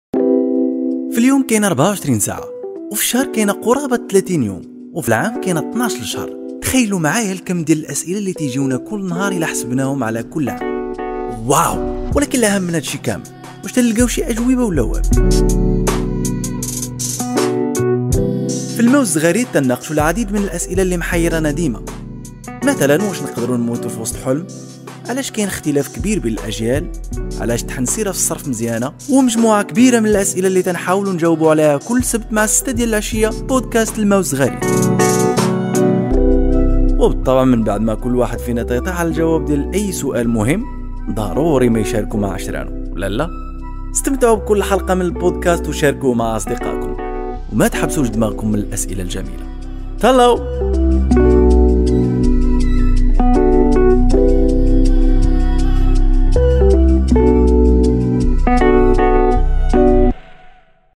تعرفو على الما و الزغاريت - بودكاست مغريبي
بودكاست اسبوعي سميتو "الما و الزغاريت" كايتعرض فيوتيوب الهدف ديالو هو مشاركة تجارب و أفكار الناس فكَـاع المجالات
Powered by MahfWorks :
سمعو لينا فأي تطبيق دالبودكاست